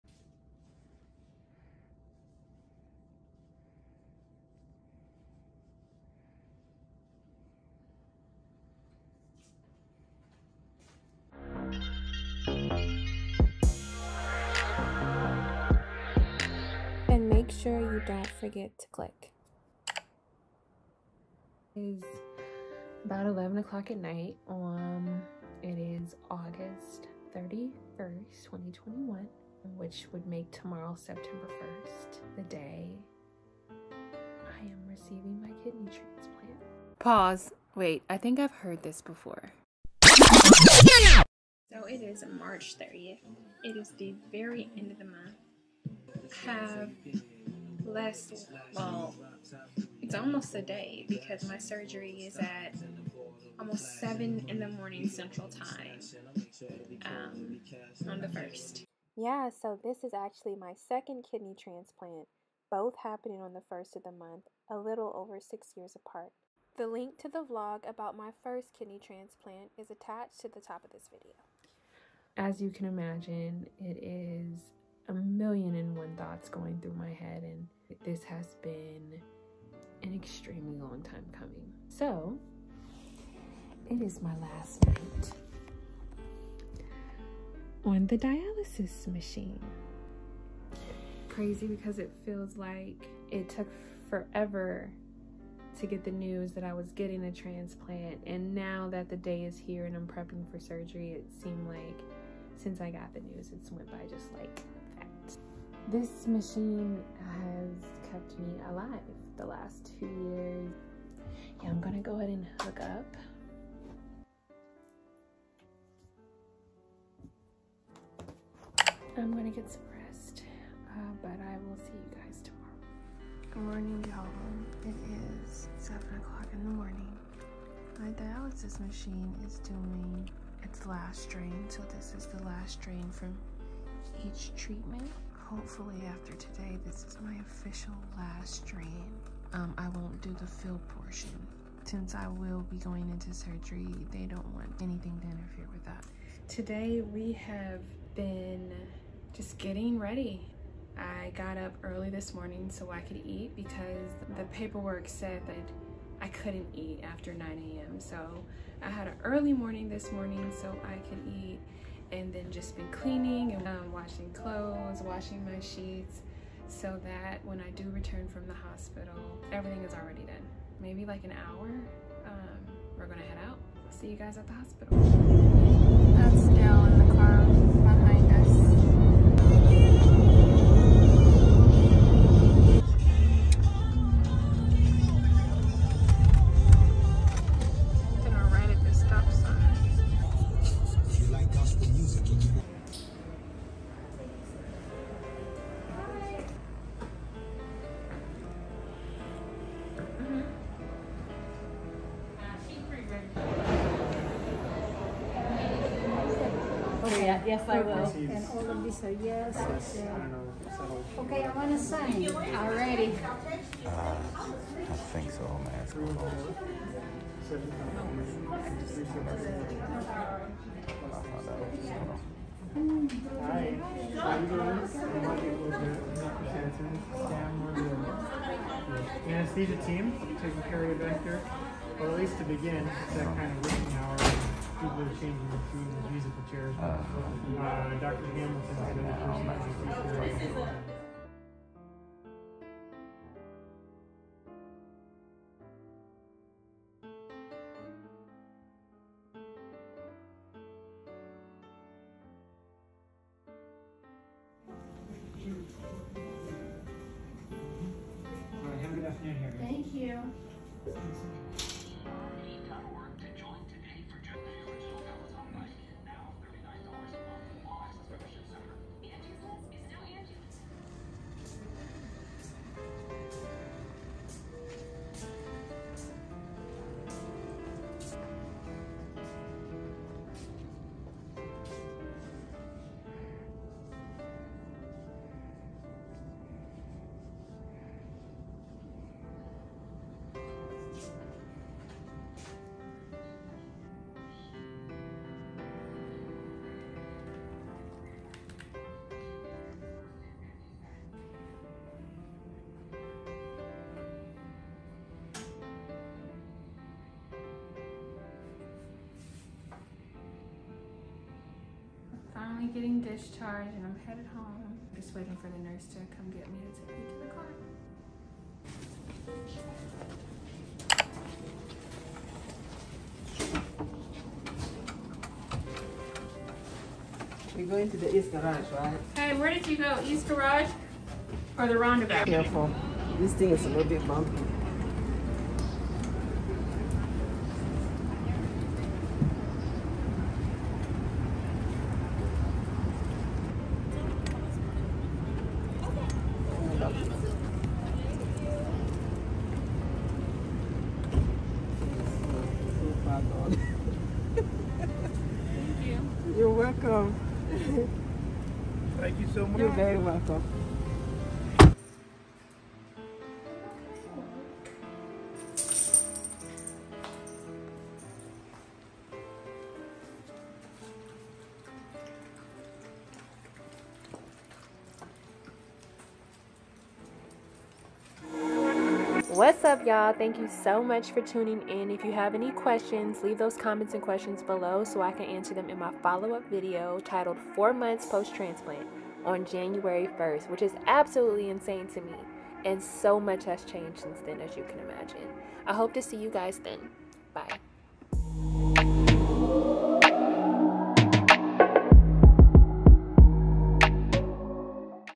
I RECEIVED MY KIDNEY TRANSPLANT!!! | VLOG
On September 1st 2021, I finally received my second (LIVING DONOR) kidney transplant so that means: NO MORE DIALYSIS. SO thankful to the doctors and nurses who took amazing care of me and most importantly to the participants of the PAIRED EXCHANGE PROGRAM that made my transplant possible.

January 1st (New Year's Day) will be my 4 month anniversary. Update video coming next week.